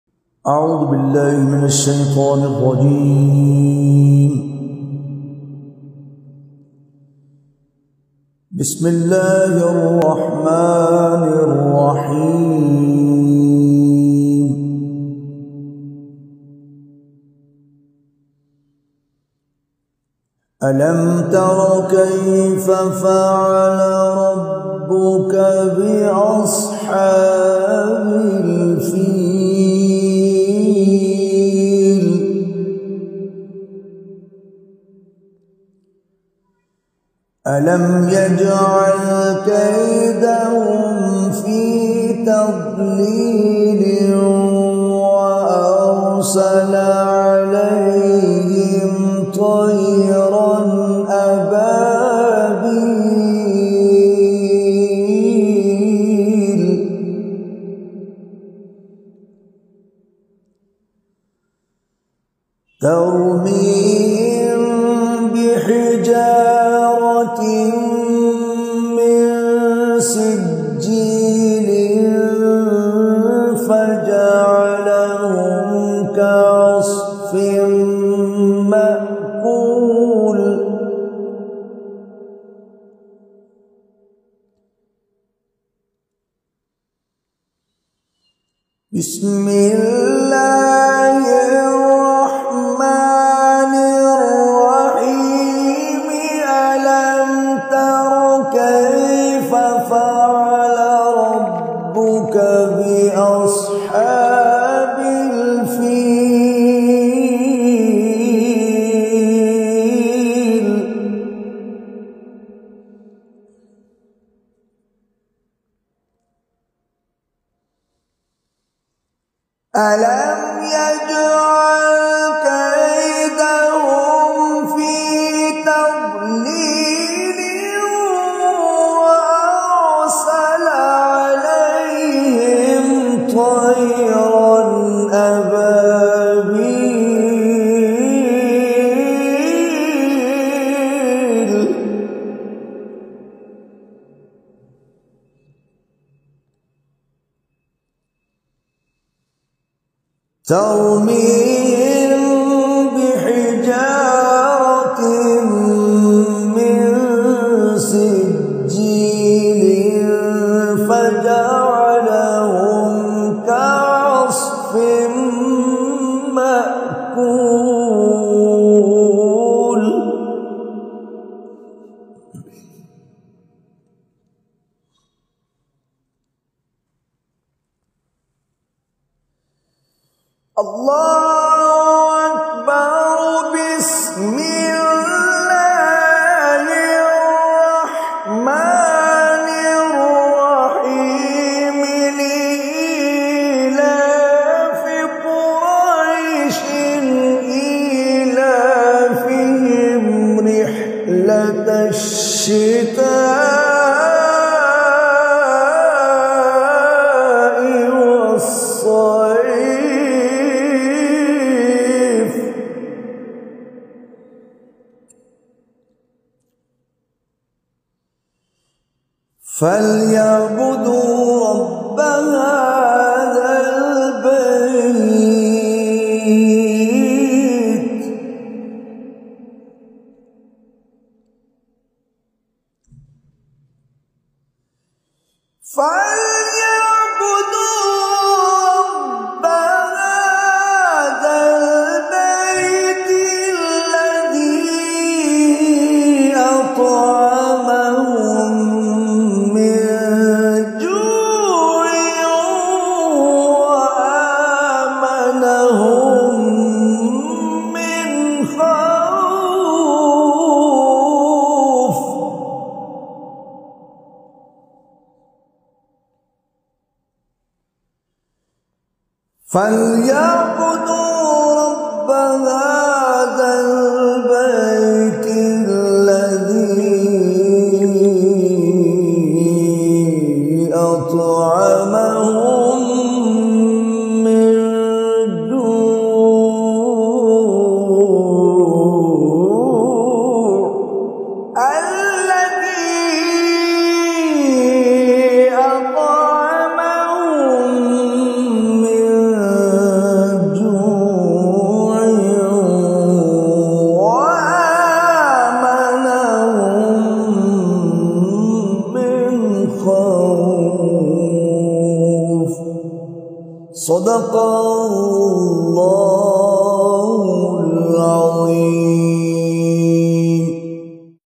World's best quran recitation || Qari Sadaqat Ali || Tilawat e Quran e pak
Beautiful Recitation of Quran e pak by Qari Sadaqat Ali sahab